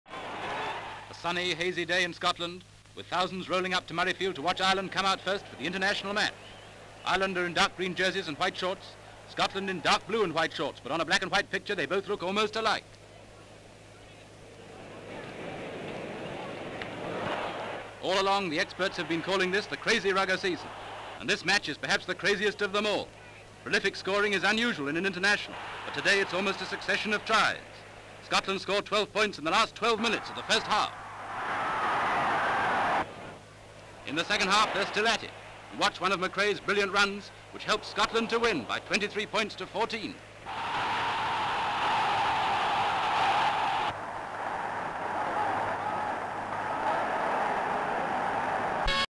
Ireland V Scotland Rugby At Murray Field Lner (1938)
Edinburgh, Scotland.

Brief high angle shot of crowds arriving at Murray Field for the International Rugby match between Ireland (slightly lighter jerseys) and Scotland.  The teams come running out of the tunnel.  Various shots of the match in progress.  Scotland score several tries and a goal, winning the match with a score of 23 points to 14.
 FILM ID:947.25